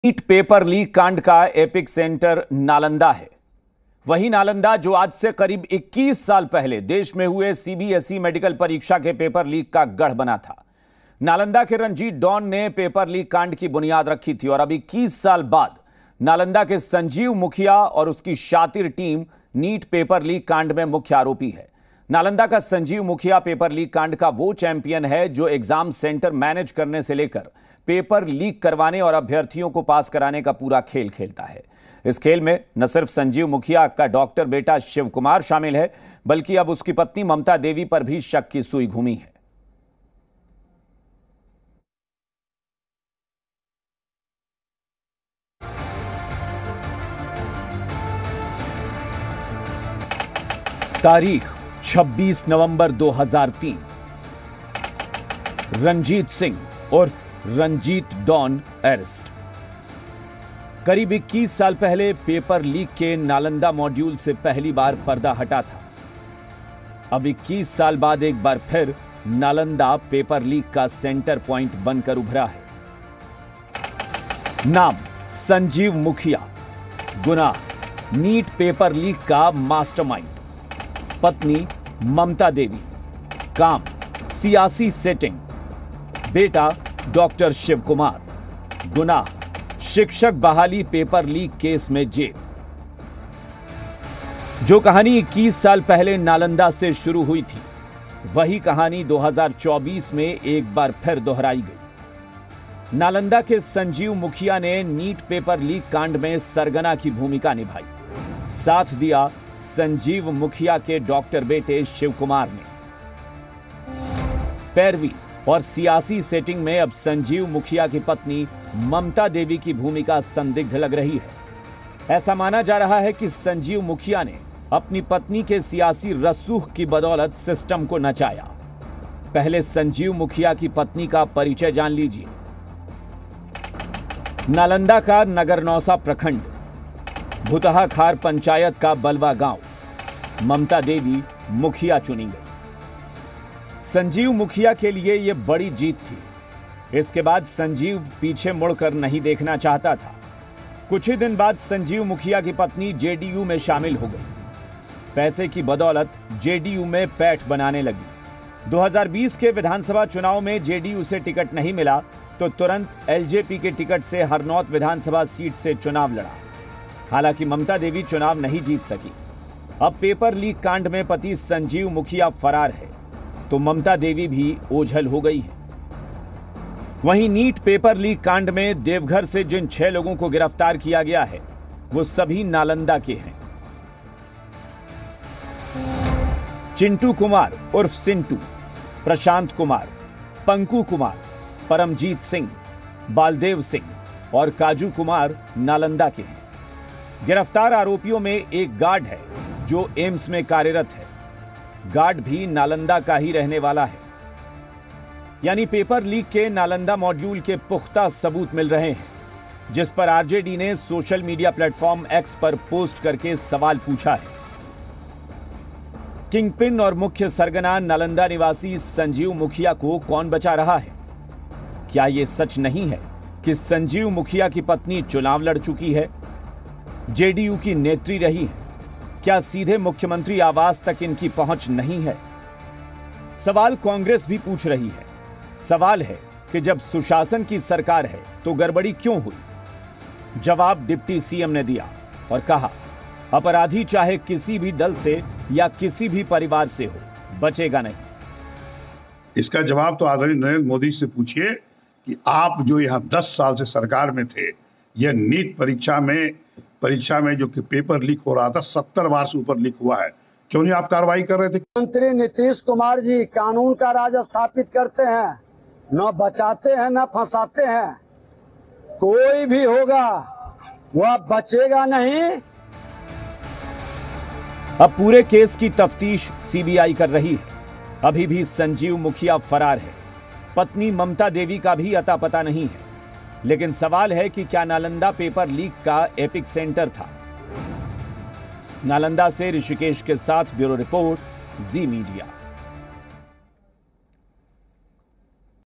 NEET Paper Leak Update: पेपर लीक का 'नालंदा मॉड्यूल' | Bihar Paper Leak | Nalanda
NEET Paper Leak Update: क्या NEET पेपर लीक कांड का एपी सेंटर नालंदा (Paper Leak Nalanda Connection) है. वहीं नालंदा. जो आज से करीब 21 साल पहले देश में हुए CBSE मेडिकल परीक्षा के पेपर लीक का गढ़ बना था. नालंदा के रंजीत डॉन ने पेपरलीक कांड की बुनियाद रखी थी. अब 21 साल बाद नालंदा के संजीव मुखिया और उसकी शातिर टीम. NEET पेपरलीक कांड में मुख्य आरोपी है...नालंदा का संजीव मुखिया. पेपरलीक कांड का वो चैंपियन है...जो एग्जाम सेंटर मैनेज करने से लेकर पेपर लीक करवाने और अभ्यर्थियों को पास कराने का पूरा खेल खेलता है...इस खेल में न सिर्फ संजीव मुखिया का डॉक्टर बेटा शिव कुमार शामिल था...बल्कि अब पत्नी ममता देवी पर भी शक की सूई घूमी है. रिपोर्ट देखिए.